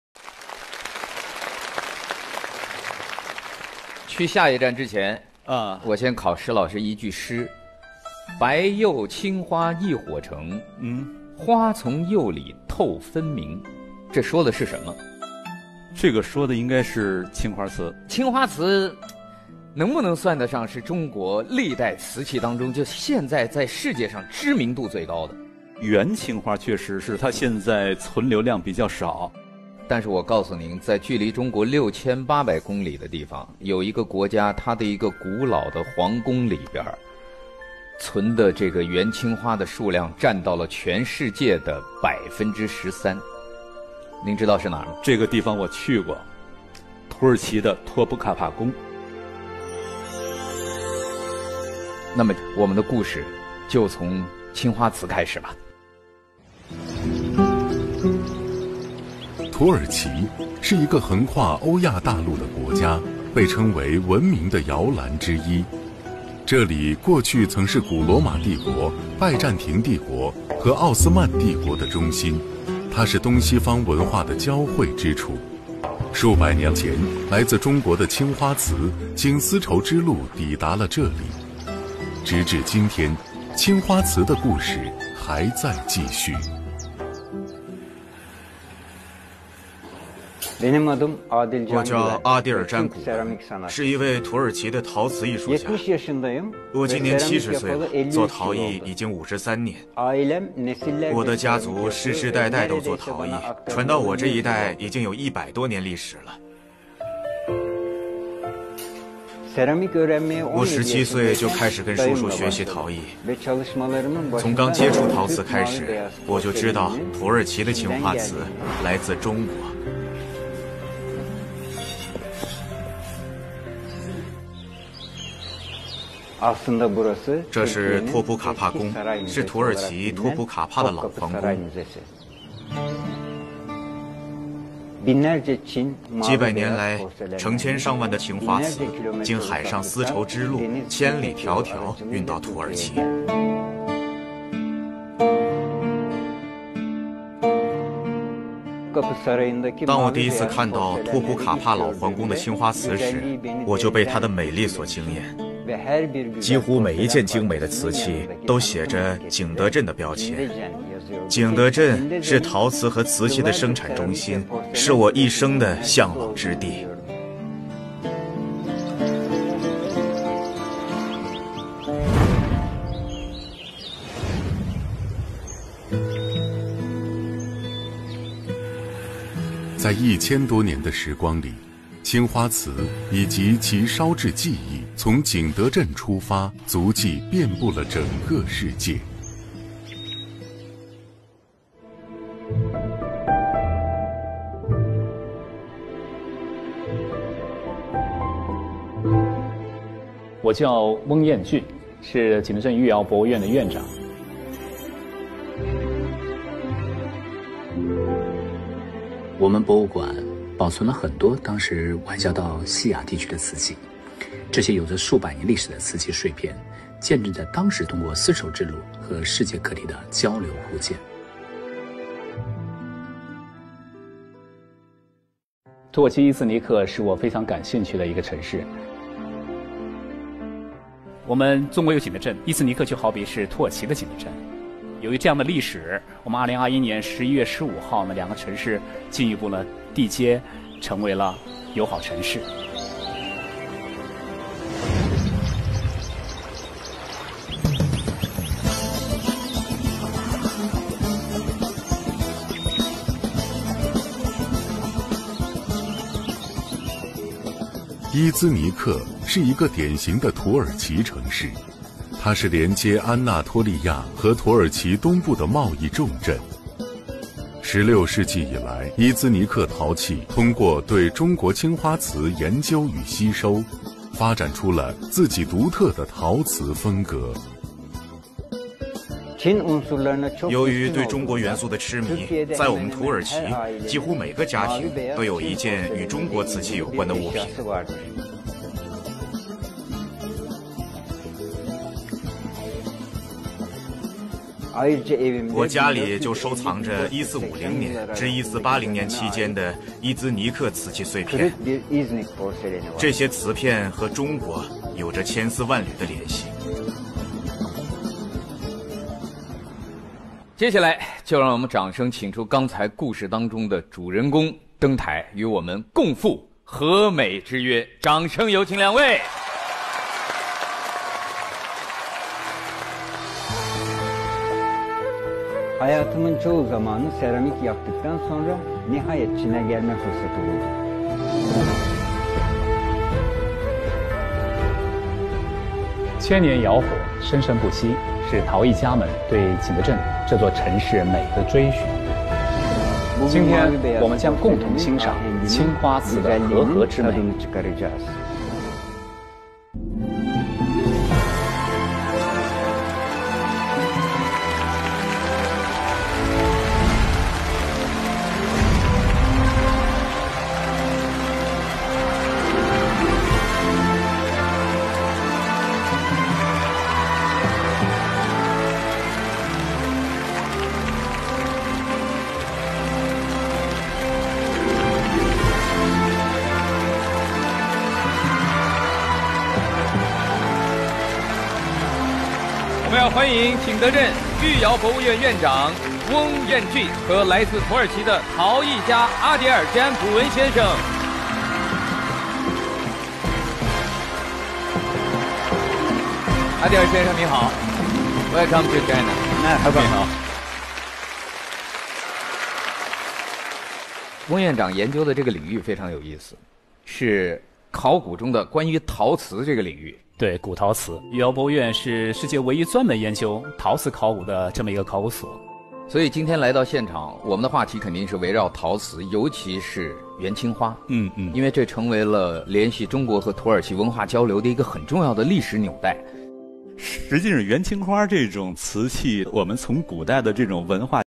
İnegöllü Adil usta Çin’in kalbini fethetti
Çin’in ilk televizyon kanalı olan CCTV 1 İznik çinisi ile Çin Çinisini konu aldığı programda İnegöllü isim olan Adil Can Güven ile  Yuyao Müzesi'nin müdürü Weng Yanjun'u  konuk aldı.
Programda İznik çinisinin özelliklerinden bahseden Adi Can Güven İznik çömlekçiliği, 16.yüzyıldan bu yana Çin seladonunun incelenmesi ve özümsenmesi yoluyla kendine özgü bir seramik tarzı geliştirdiğini belirtti.
İznik ile Çin’in Jingdezhen kentinin çömlekçilik üzere benzer bağları olduğunu vurgulayan Adil Can Güven 15 Ocak 2017 kardeş şehir protokolü imzalandığını belirtti.
Programda çömlekçiliğe 17 yaşında iken amcasından öğrenmeye başladığını belirten Adil Can Güven , Topkapı sarayında gördüğü eserlerden etkilenerek seramiğe ilgisinin arttığını belirtti.
Ailesinin de nesillerdir çömlek yaptığını belirten Güven kendisinin 70 yaşında olduğunu 53 senedir aralıksız çömlekçilik yaptığını söyledi.
BELGESELİ ÇEKİLDİ
Öte yandan Adil Can Güven’in de içinde yer aldığı çinicilik üzerine belgeselde programda yayınlandı.
Çinlilerin özel ilgisini çeken program milyonlarca kişi tarafından izlenildi.
ADİL CAN GÜVEN KİMDİR
1953 yılında Bursa İnegöl’de dünyaya gelen Adil Can Güven, 1970 yılından itibaren ustası ve dayısı Abdurrahman Özer ile birlikte Kütahya, Çanakkale, ve Çan’da eski seramik ustalarının derslerine katılarak ustalardan el aldı. 1983 yılında ustasının vasiyeti üzerine eşi Nurhayat Hanım’la beraber İnegöl’de atölyesini açtı. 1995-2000 yılları arasında İznik Meslek Yüksekokulu’nda öğretim görevlisi olarak çalıştı. 1998 yılında İznik’e taşıdığı atölyesinde oğulları Rauf ve Cem’le birlikte çalışmalarına devam ediyor.